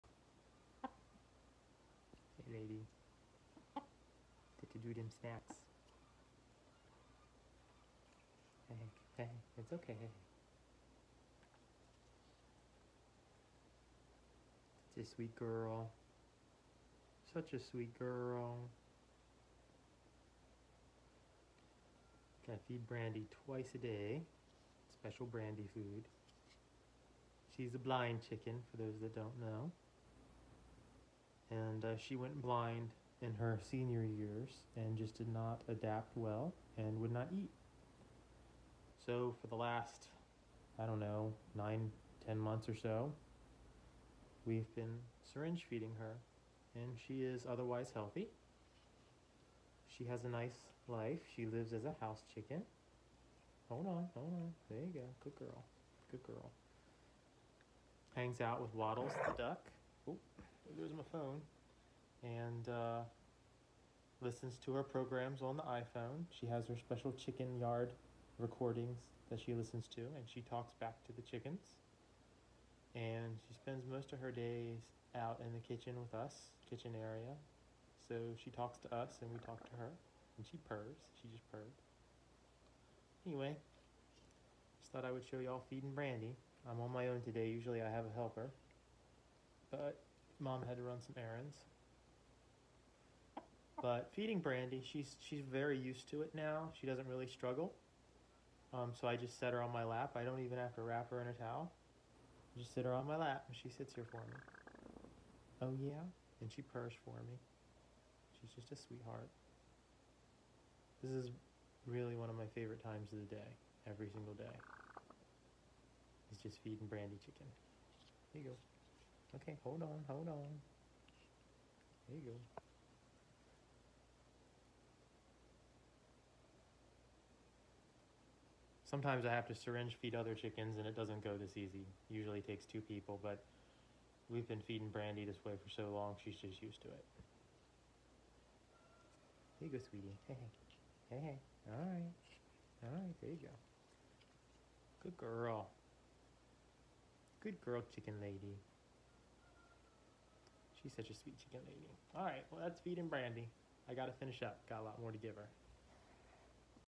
Syringe Feeding Brandee The Blind Chicken
Brandee Is a elderly blind chicken. She went blind from old age but is otherwise healthy and we have to feed her with a syringe twice per day. She lives a happy life in the house along with waddles the Peking duck.￼